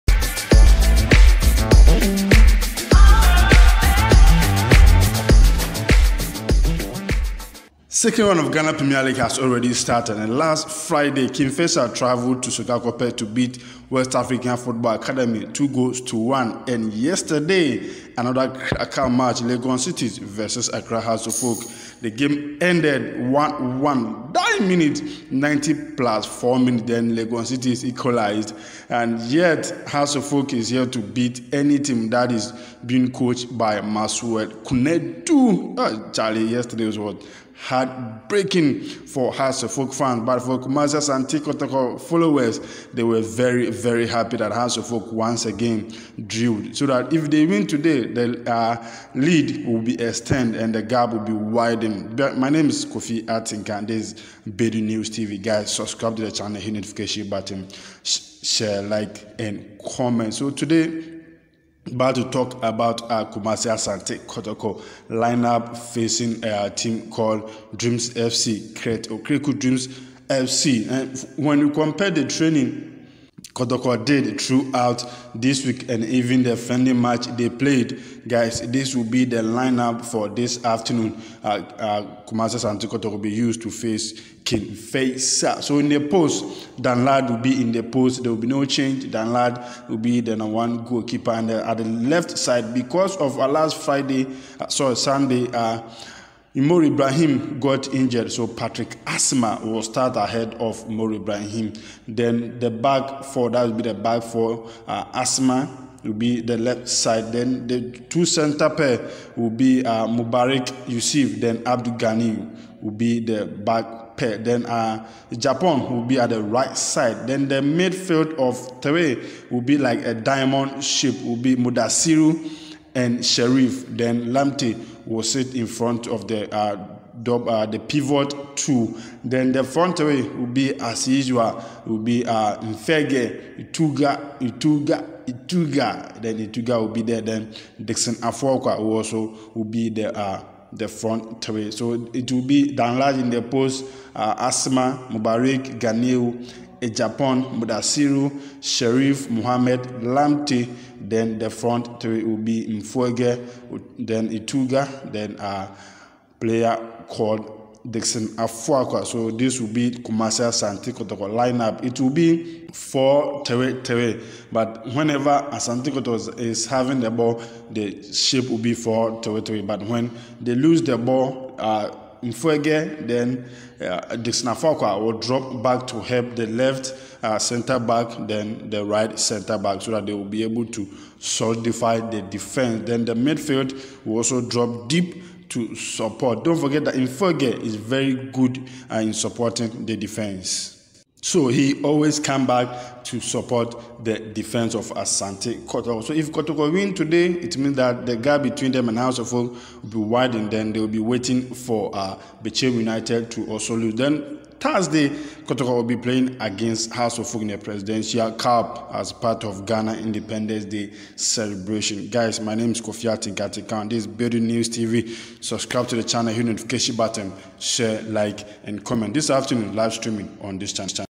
This Is How Kotoko Will Lineup To Face Dreams FC
Kotoko is going into this game as favourites and also league leaders. Kotoko used 6 minutes to beat dreams FC in the first round and they will seek to do the same today